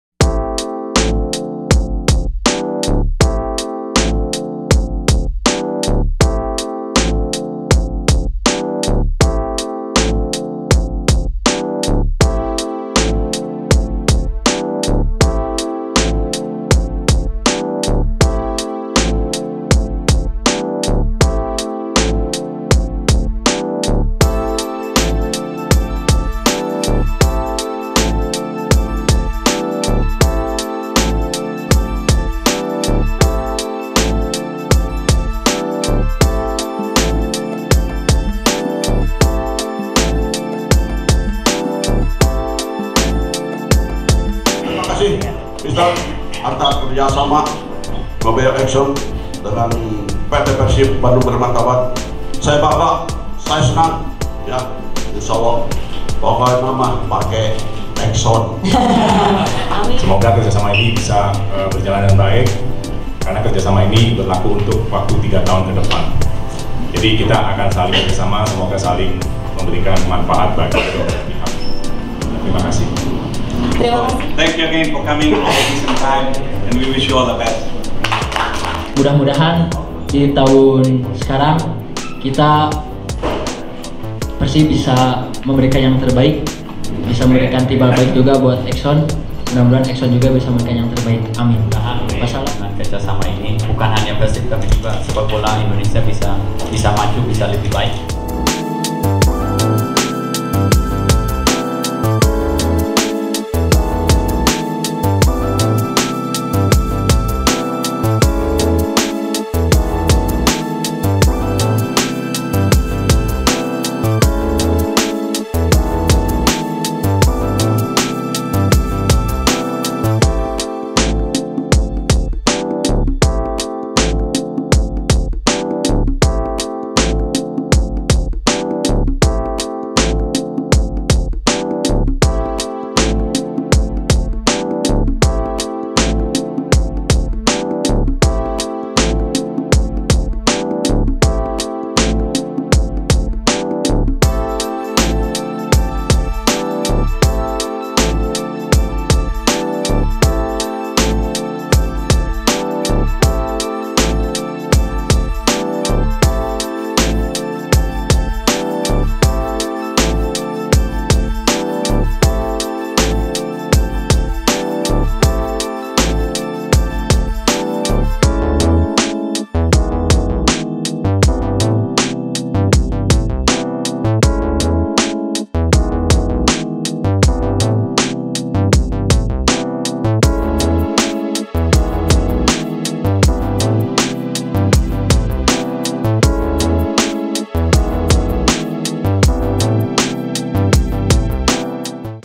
MOBIL RESMI JADI MITRA PERSIB
ExxonMobil dengan produknya, Mobil, resmi menjalin kerja sama dengan dengan PERSIB sebagai mitra resmi untuk tahun 2019. Dalam sesi konferensi pers di 1933 Dapur & Kopi, Jalan Sulanjana, Bandung, Kamis 11 April 2019, Wakil Presiden ExxonMobil, Kamal Singh mengaku senang dengan kerjasama ini.